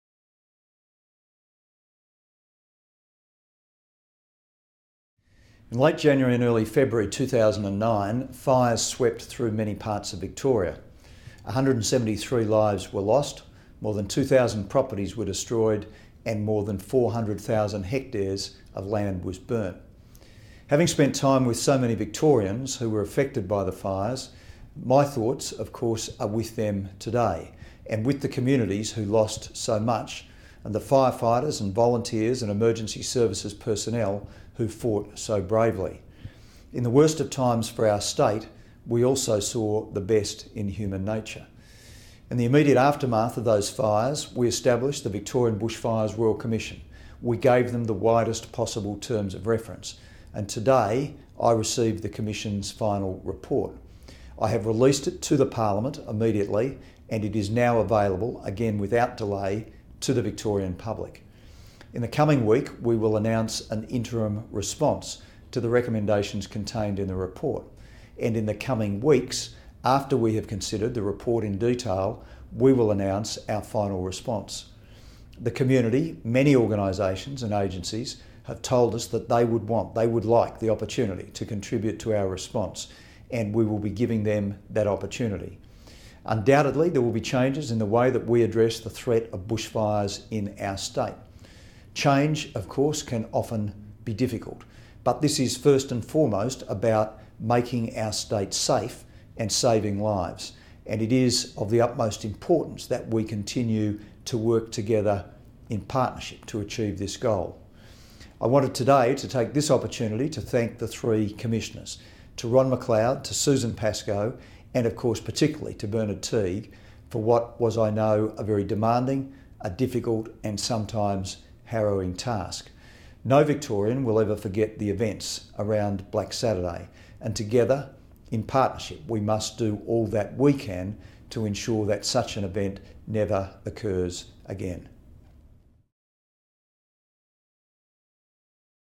John Brumby - Final Report of the 2009 Victorian Bushfires Royal Commission
Premier John Brumby speaks about the 2009 Victorian Bushfires Royal Commission final report.

Transcript

In late January and early February 2009, fires swept through many parts of Victoria.

173 lives were lost.  More than 2000 properties were destroyed and more than 40,000 hectares of land was burnt.

Having spent time with so many Victorians who were affected by the fires, my thoughts of course, are with them today and with the communities who lost so much, and the firefighters and volunteers and emergency services personnel who fought so bravely.

In the worst of times for our State, we also saw the best in human nature.

In the immediate aftermath of those fires, we established the Victorian Bushfires Royal Commission, we gave them the widest possible terms of reference.

And today, I received the Commission's final report.

I have released it to the Parliament immediately and it is now available, again without delay, to the Victorian public.

In the coming week, we will announce an interim response to the recommendations contained in the report.

And in the coming weeks, after we have considered the report in detail, we will announce our final response.

The community, many organisations and agencies have told us that they would want, they would like, the opportunity to contribute to our response -- and we will be giving them that opportunity.

Undoubtedly, there will be changes in the way we address the threat of bushfires in our State.

Change of course can often be difficult.

But this is first and foremost about making our State safe and saving lives -- and it is of the utmost importance that we continue to work together in partnership to achieve this goal.

I wanted today to take this opportunity to thank the three Commissioners --  to Ron McLeod, to Susan Pascoe, and of course particularly to Bernard Teague -- for what was I know a very demanding a difficult and sometimes harrowing task.

No Victorian will ever forget the events around Black Saturday.

And together, in partnership, we must do all we can to ensure such an event never occurs again.